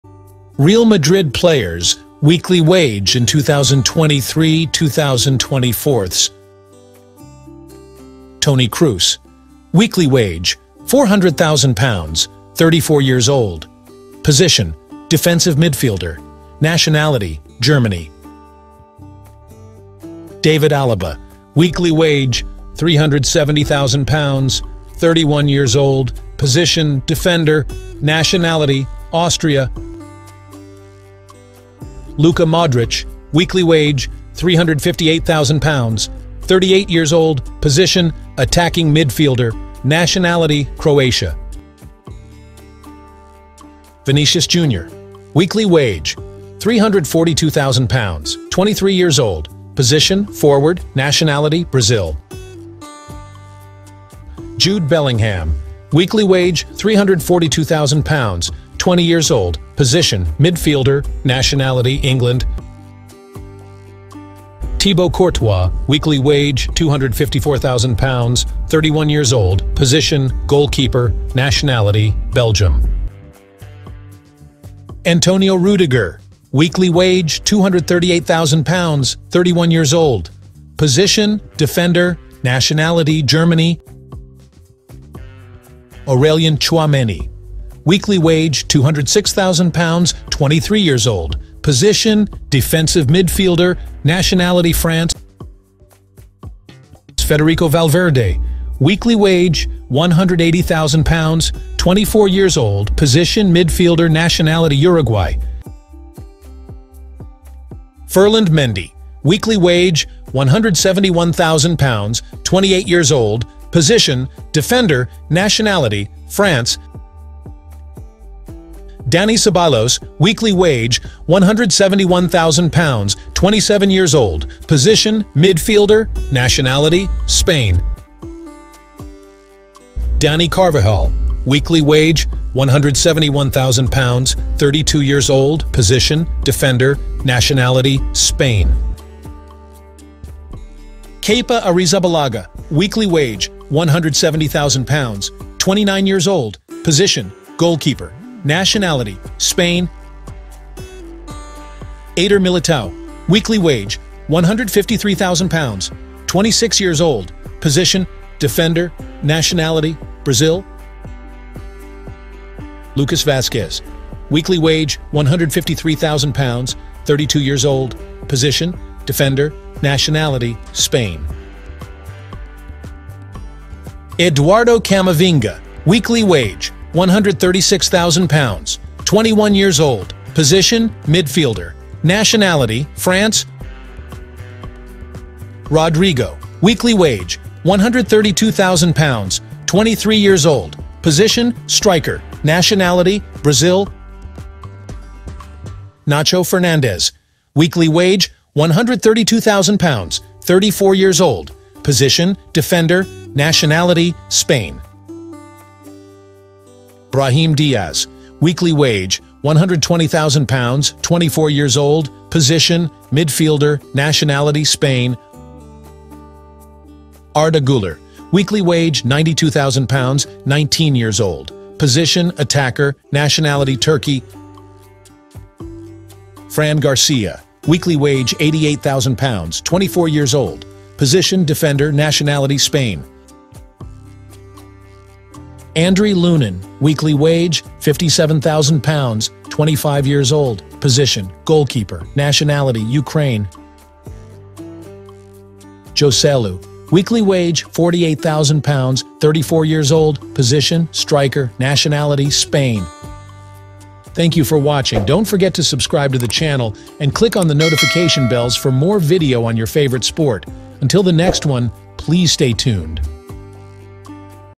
REAL MADRID PLAYERS SALARY SEASON 2023/2024
Real Madrid (La Liga) Total Gross Salaries for the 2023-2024 Season is €289,940,000, or €5,575,769 per week. The Highest Paid Player at Real Madrid is Toni Kroos (Central Midfield) with a Gross Annual Salary of €24,380,000, or €468,846 per week. Toni Kroos, who is 34 years old, has 1 year and €24,380,000 gross remaining with Real Madrid.
All salary figures are estimates as actual salaries may vary.

Toni Kroos
David Alaba
Luka Modric
Vinicius Junior
Jude Bellingham
Federico Valverde
Thibaut Courtois
Antonio Rüdiger
Aurélien Tchouaméni
Rodrygo
Eduardo Camavinga
Dani Ceballos
Daniel Carvajal
Ferland Mendy
Lucas Vázquez
Kepa Arrizabalaga
Nacho Fernández
Brahim Díaz
Éder Militão
Fran García
Arda Güler
Andriy Lunin
Joselu